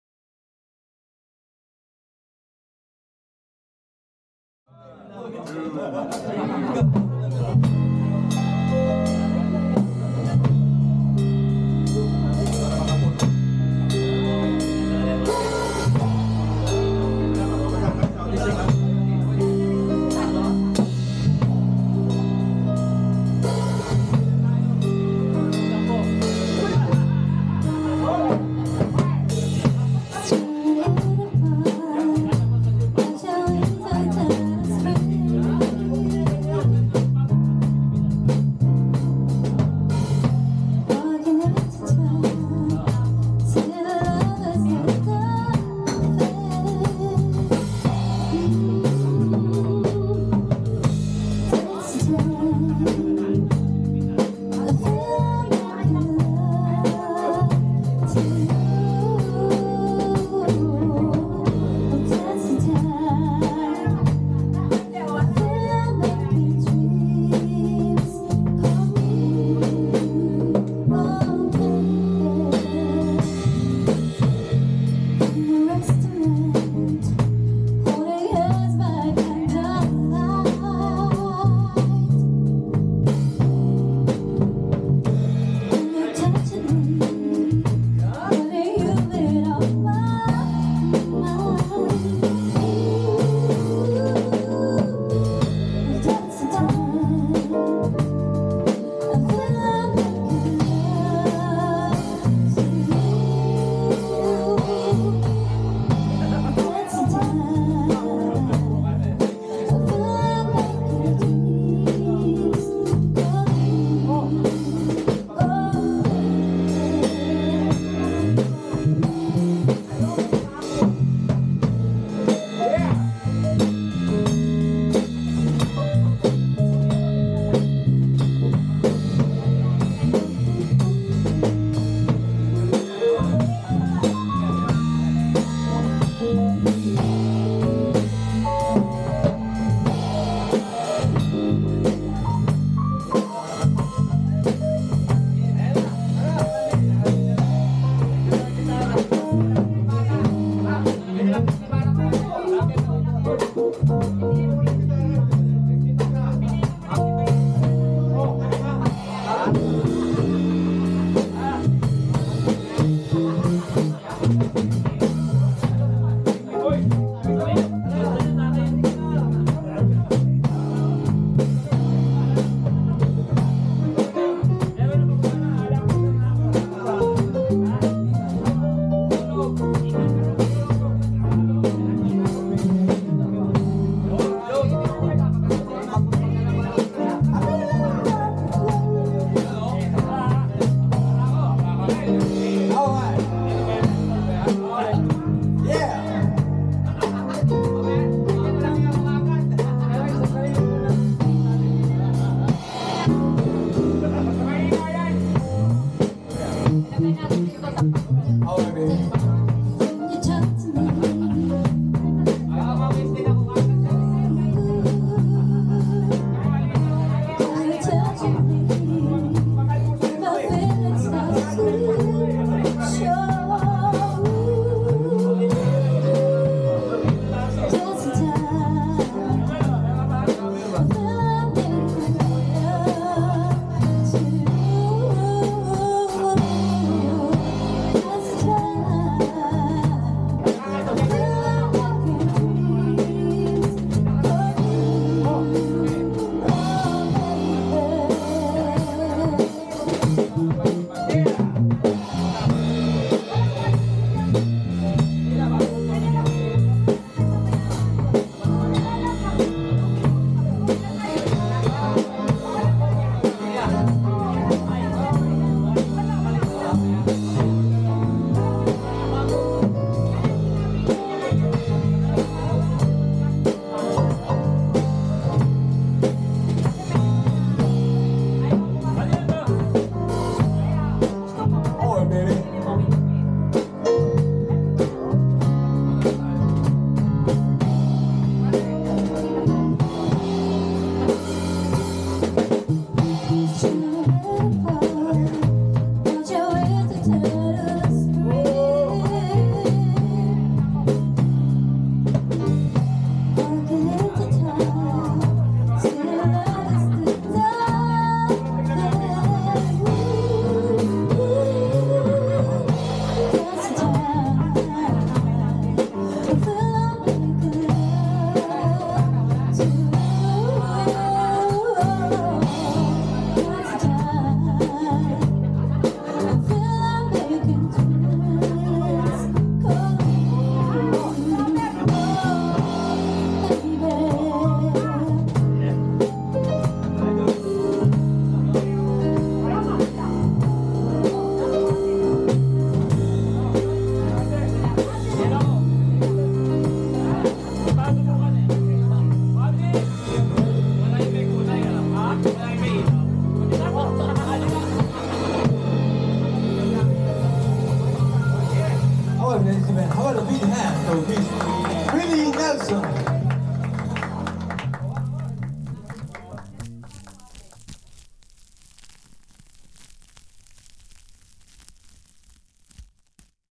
Alex Ebarle, Jose Alba, Ashley, Leo, Dan Vito / Filipino Musicians Japan Get Together at Bamboo
Alex Ebarle, Jose Alba, Ashley Rosaldo, Harley Hal Lequinne, Dan Vito